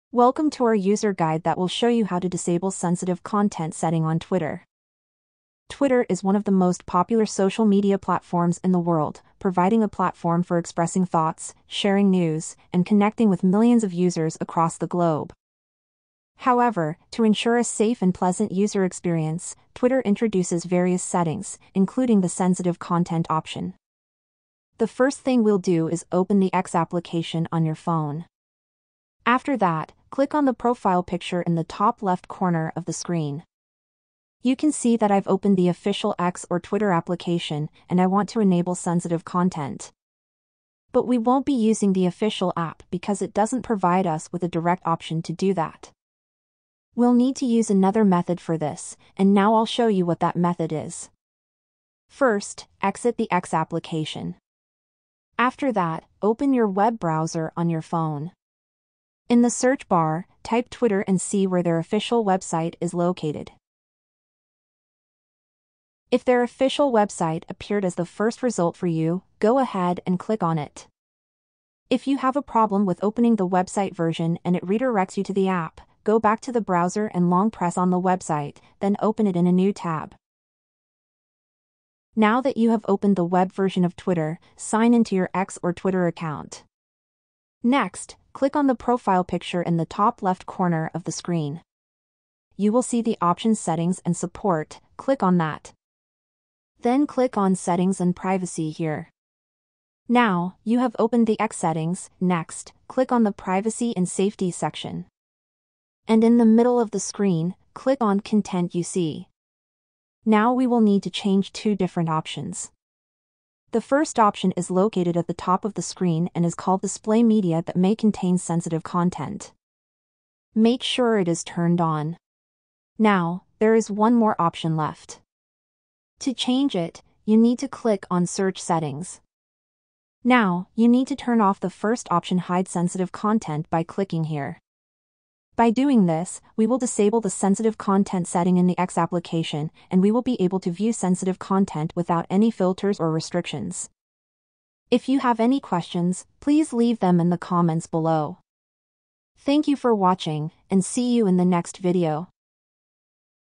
How To Turn Off X Twitter Sensitive Content Setting
Do you want to turn off the sensitive content filter in the X app? I'm gonna show you how to enable the sensitive content on (X) Twitter so you can see violent, graphic and other type of content. Make sure to watch the video until the end so you don't make any mistakes.